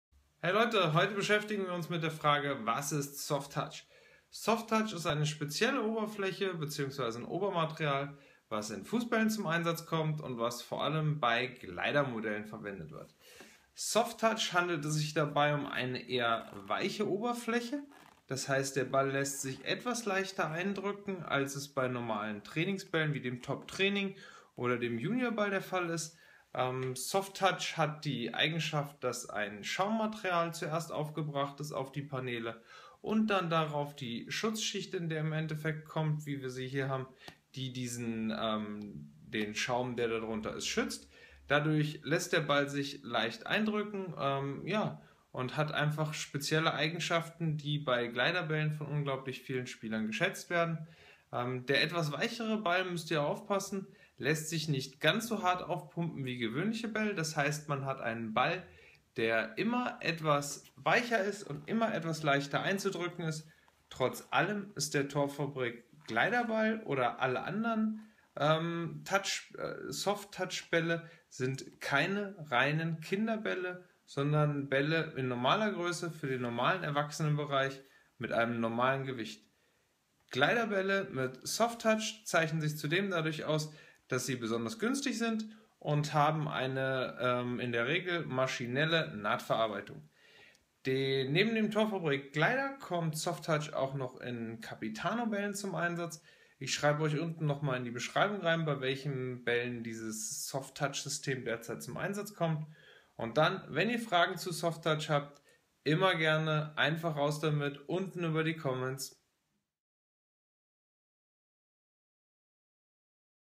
Soft Touch (Adidas Fußbälle) Glider / Captiano
Der Adidas Torfabrik Glider und der Adidas Finale Capitano haben mit "Soft Touch" eine andere Oberfläche wie alle anderen Adidas Fußball. Im Video geht es um genau diese "Soft Touch" Eigenschaften der Trainingsfußbälle.

Wozu ist "Soft Touch" da und was bringt es bei den Adidas Torfabrik und Adidas Finale Fußbällen.